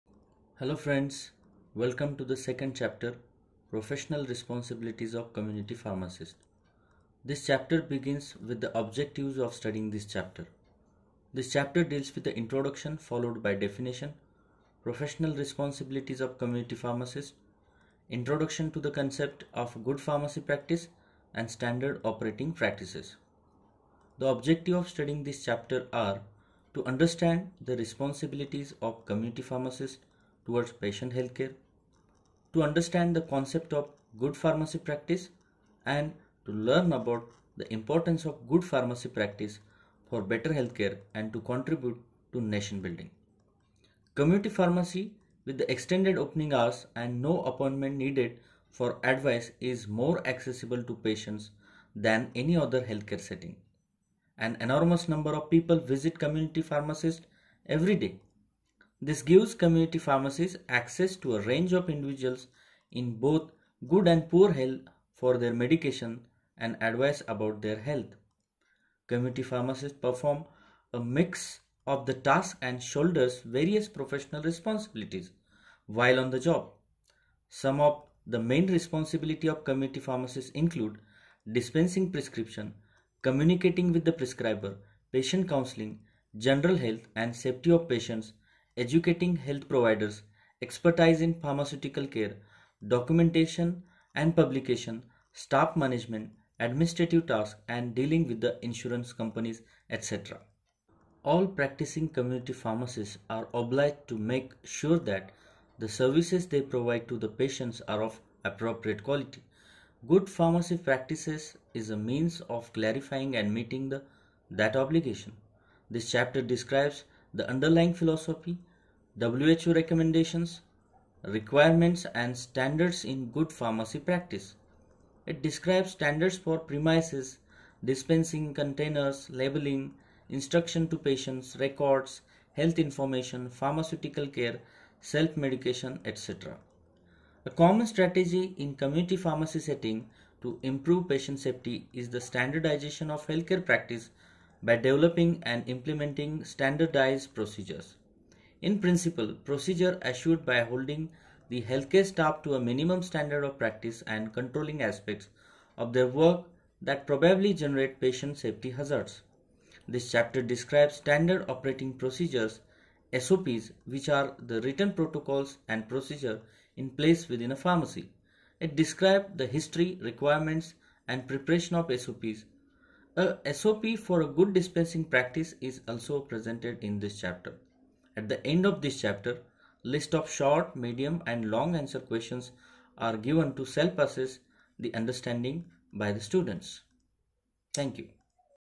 Chapter 2 - Professional Responsibilities of Community Pharmacists | SY Diploma Pharmacy | Nirali
In this Video, Professor Dr. Ashok Hajare explains the second chapter of the Book on Community Pharmacy and Management to the students of Second Year Diploma in Pharmacy.

This video is based on the Textbook published by Nirali Prakashan.